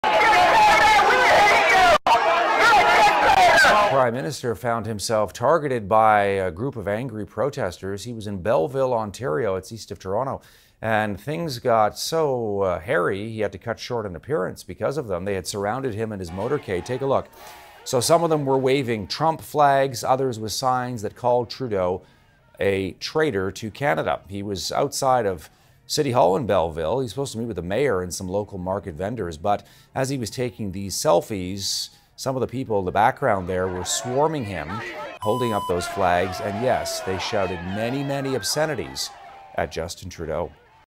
Justin Trudeau swarmed by protesters in Belleville, Ontario
Prime Minister Justin Trudeau was swarmed by angry protesters at a stop in Ontario.